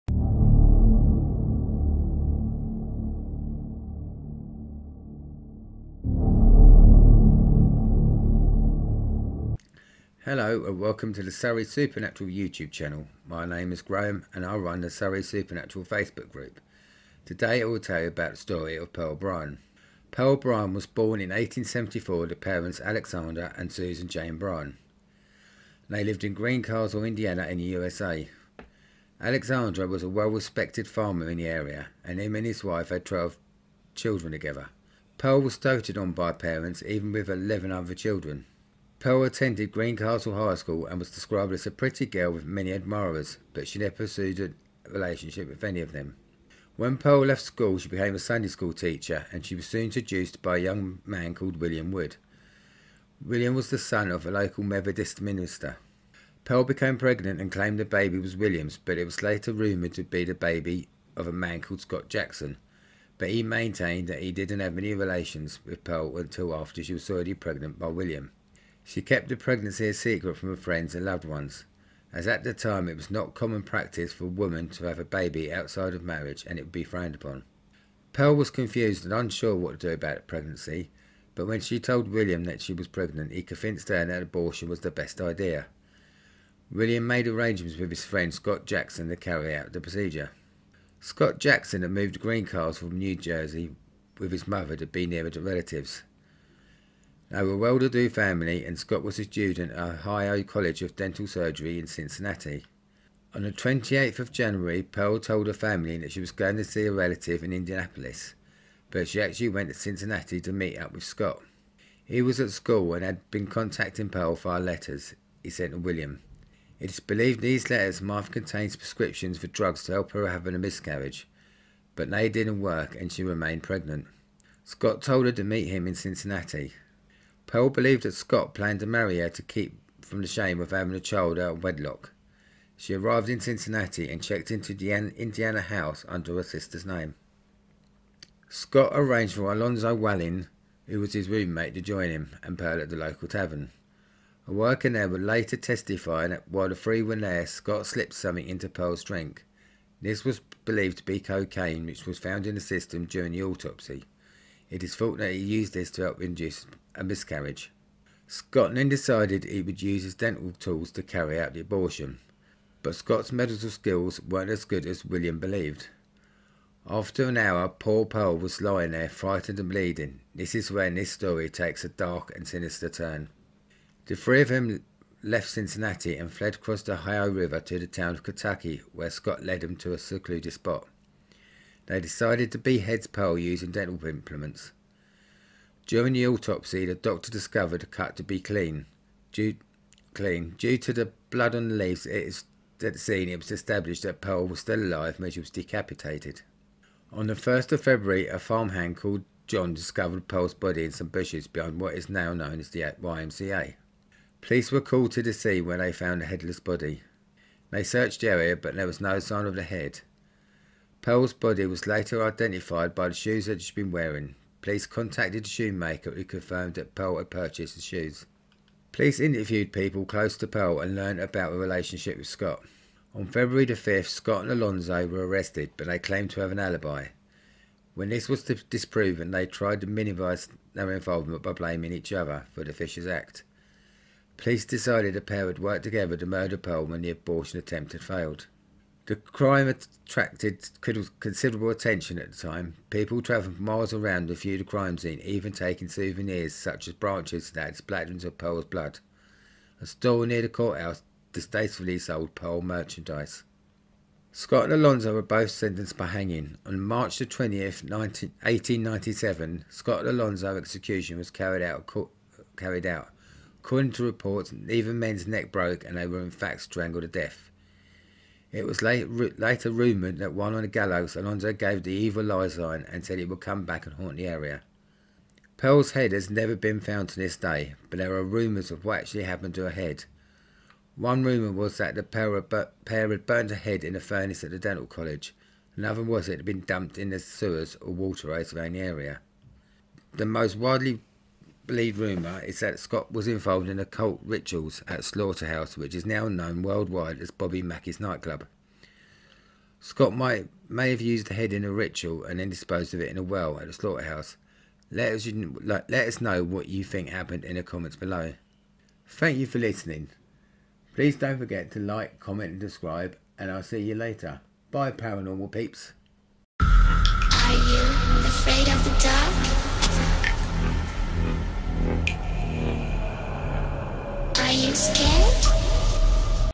The Chilling Case of Pearl Bryan
This  is  the story of Pearl Bryan who was viciously Murdered back in the 1800's

Sources